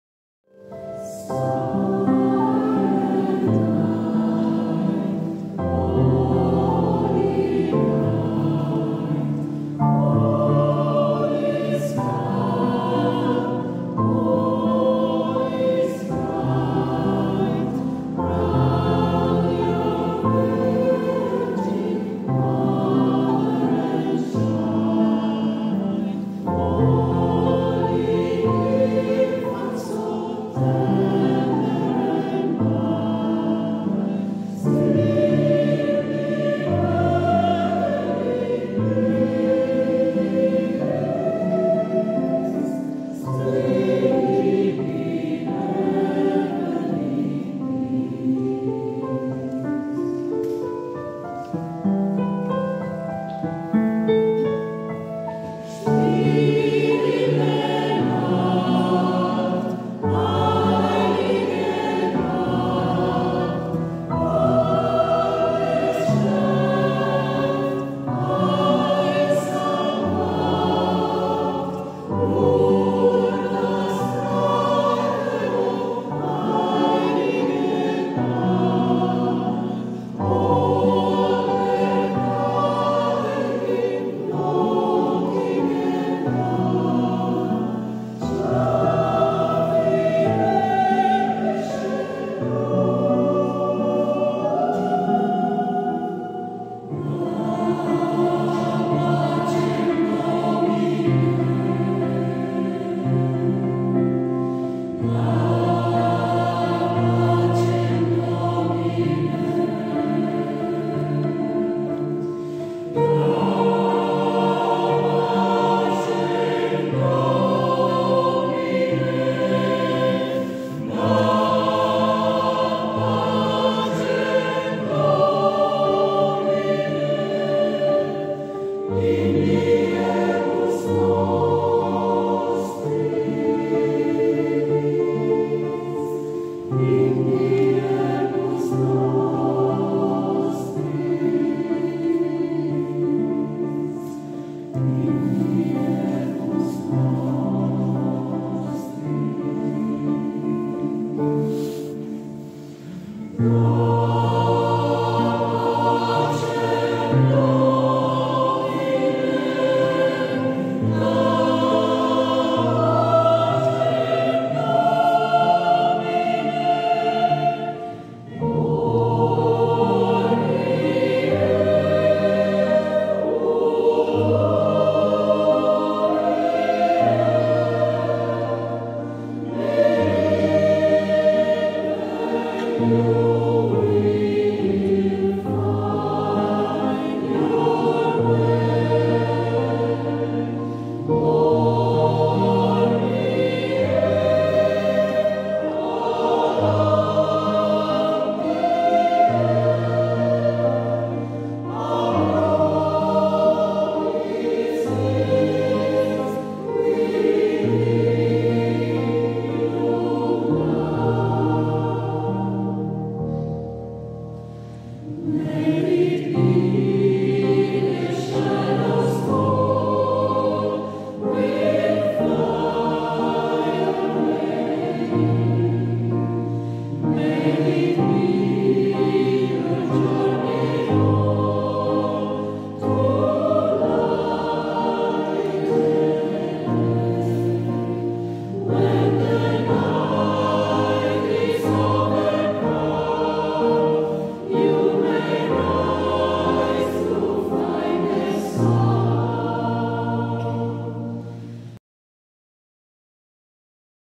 Corale San Jacopo
Il concerto della Corale San Jacopo alla Pieve di Cascia, il 23 dicembre 2022.